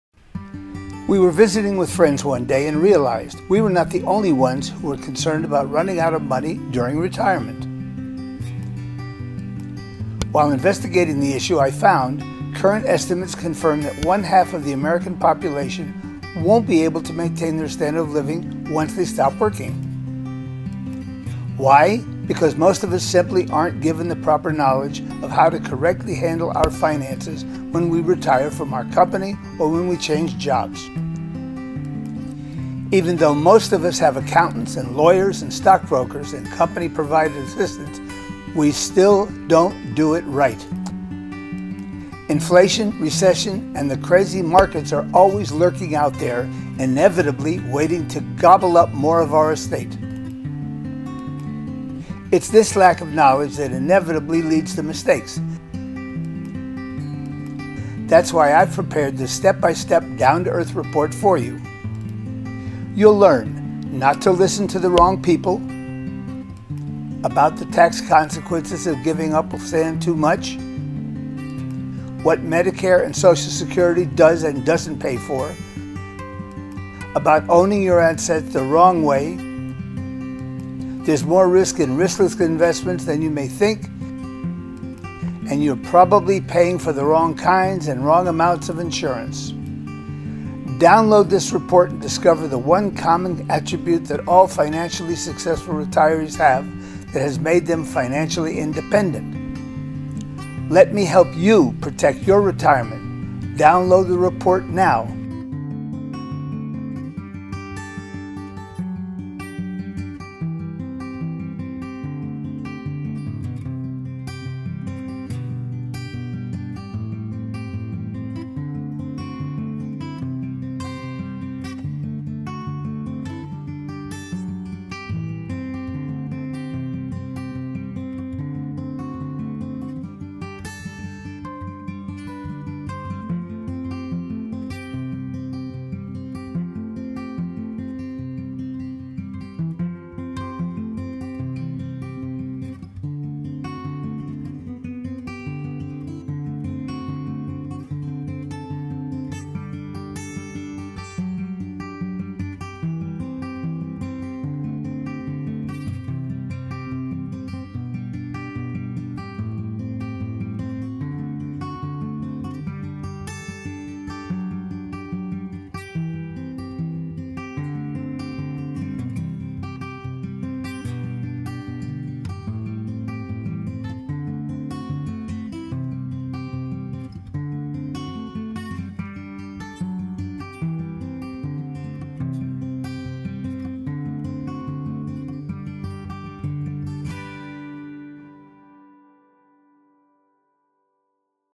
How To Avoid Current Retirement Planning Mistakes
This report will help keep your retirement savings plan from going down the drain.